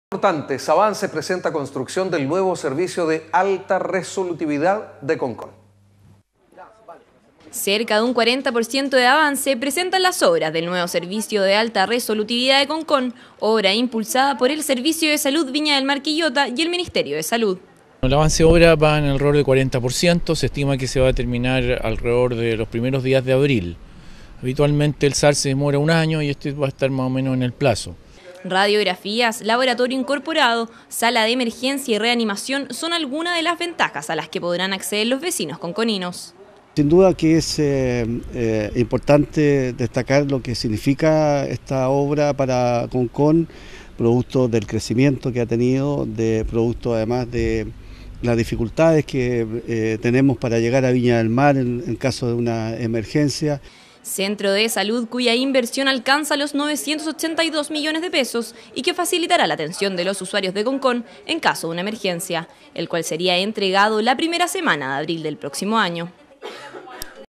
QV NOTICIAS - AVANCES SAR CONCÓN
SERVICIO DE ALTA RESOLUTIVITAD (SAR) DE CONCÓN AVANZA EN UN 40% DE CONSTRUCCIÓN.

• Nuevo dispositivo de atención primaria de urgencia, que construye el Servicio de Salud Viña del Mar Quillota y MINSAL, permitirá atender con mayor resolución las emergencias de los habitantes de la comuna de Concón y sus alrededores.